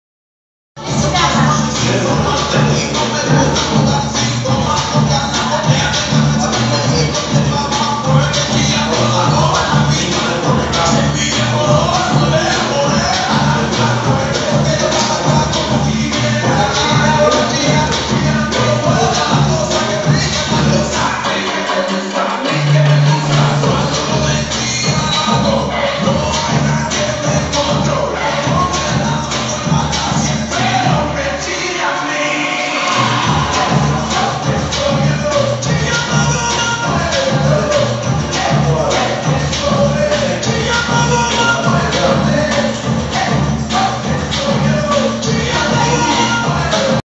Rouslan Aliev just practicing
A few steps in Samba... :)